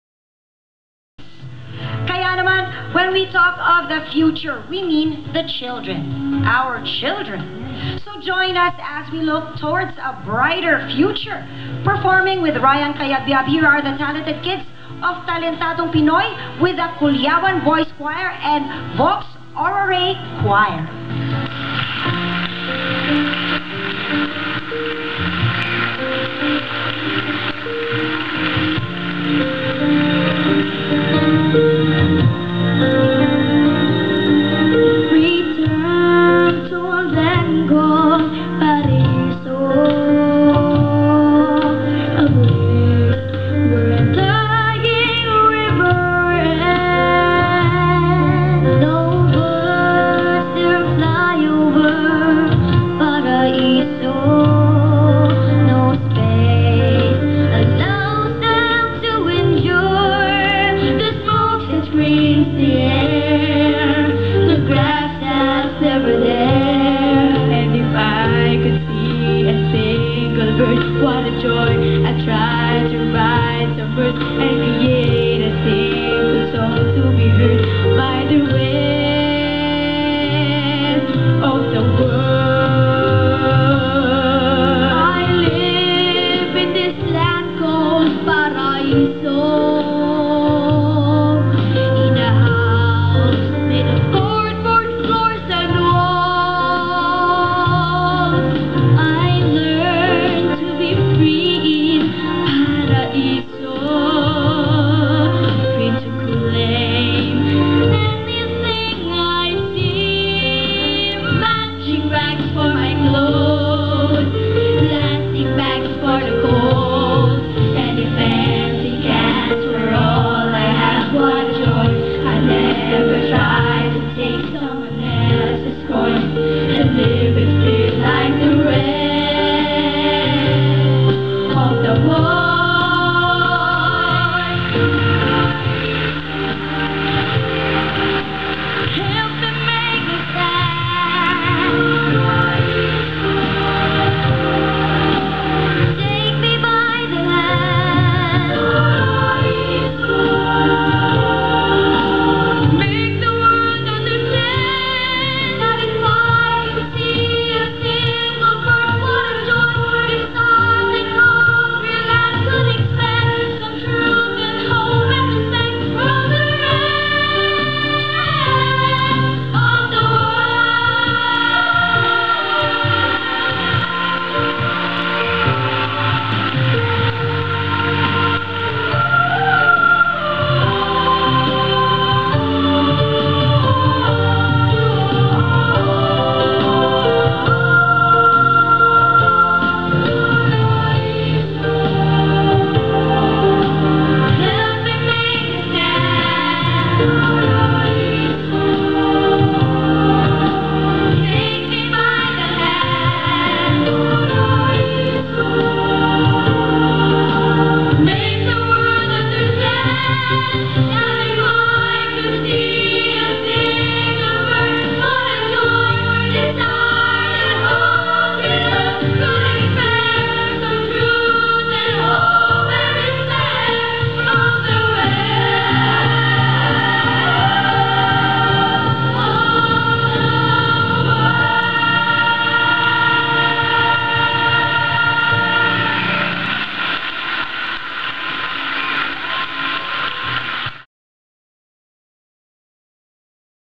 GO5 Launch - Sir Ryan Cayabyab with Chloe, JC, Melea, Alyssa and Mark (TP-KIDS)
Go 5 Trade Launch at the Resorts World - Mr. Ryan Cayabyab with Chloe, Melea, Alyssa, JC and Mark (Talentadong Pinoy Kids)  --- Thank you po TV5 we all enjoyed performing!